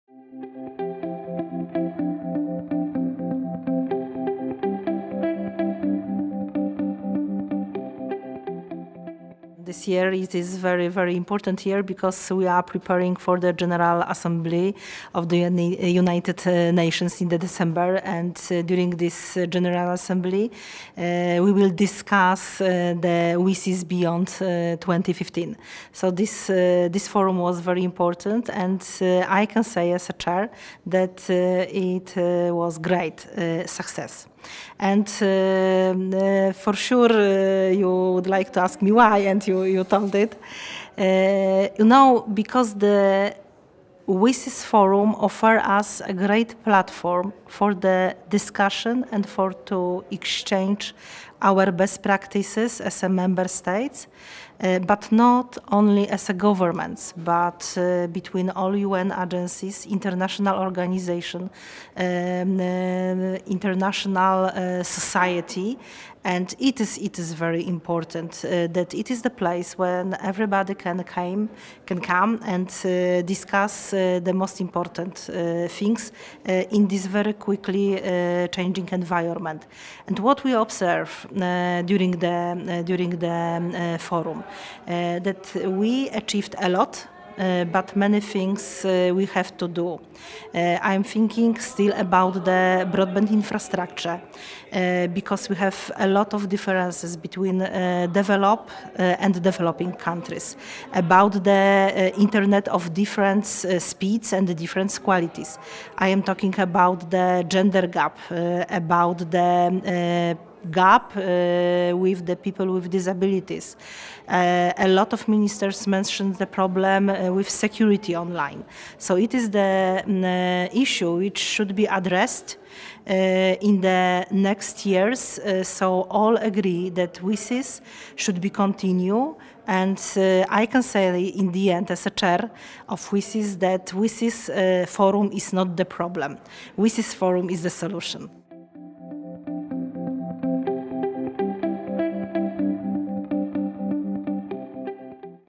WSIS FORUM 2015 INTERVIEWS: Magdalena Gaj, Chair, WSIS Forum 2015
Interview with Magdalena Gaj, Attourney, Supreme Court of India WSIS FORUM 2015, Geneva, Switzerland. 25-29 May 2015.